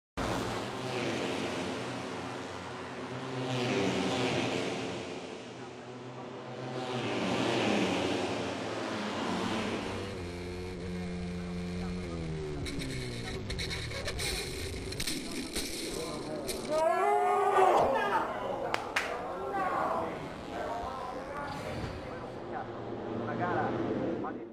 Romano Fenati Crash at San Marino Circuit
Romano Fenati Crashes out of a three-second lead! at San Marino Circuit